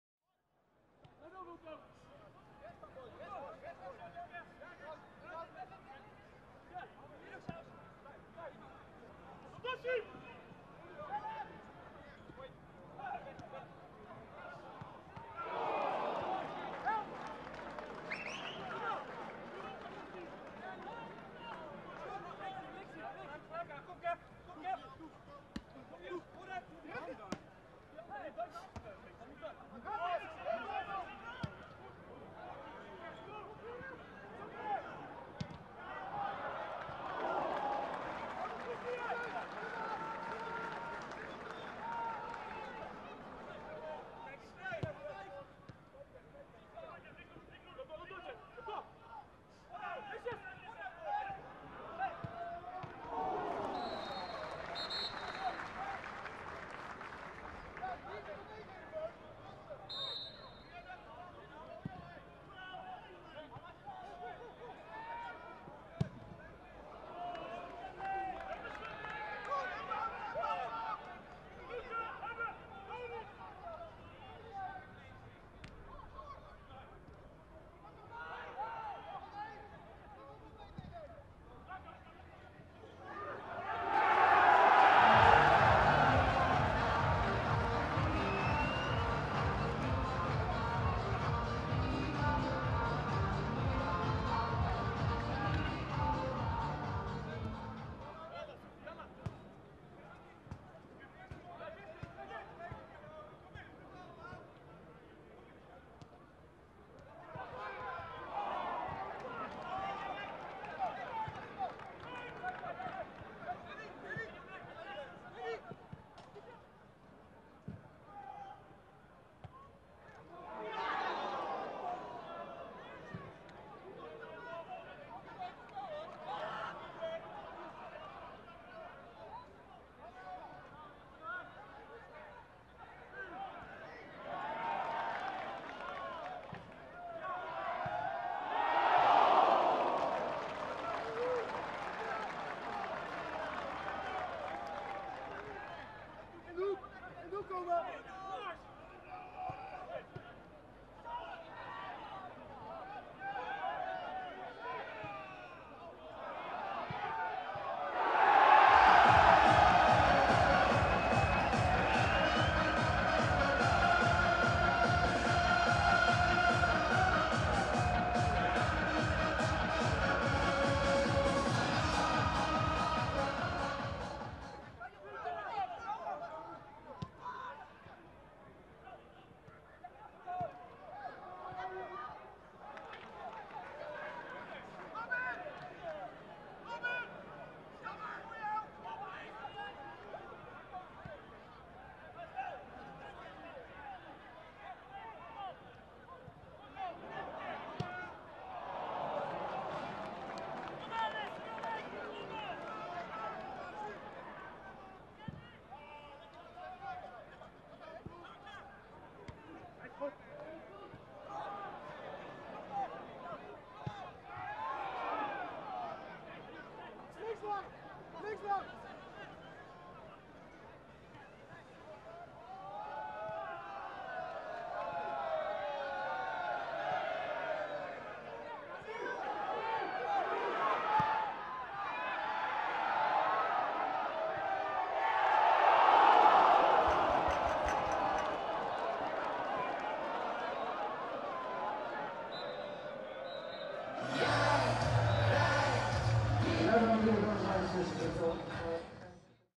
[17/18] Samenvatting G.V.V.V. - FC Lisse | GVVV TV
De beelden van de Tweede Divisie-wedstrijd tussen G.V.V.V. en FC Lisse, gespeeld op zaterdag 10 maart 2018.